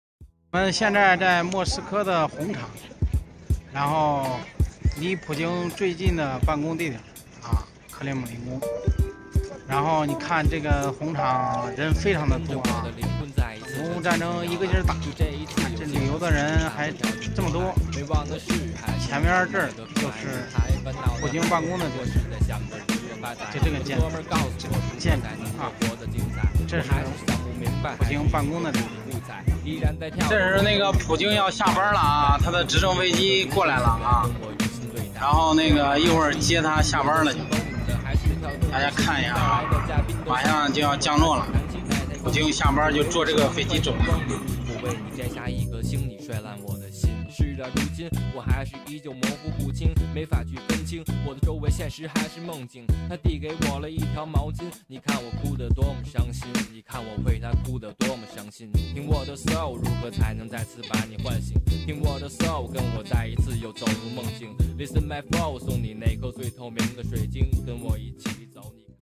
See Putin off duty in Kremlin's Red Square
I came to visit the Kremlin and unexpectedly saw Putin’s helicopter coming to pick him up.
Amanda Group - One-stop water treatment product supplier,since 1997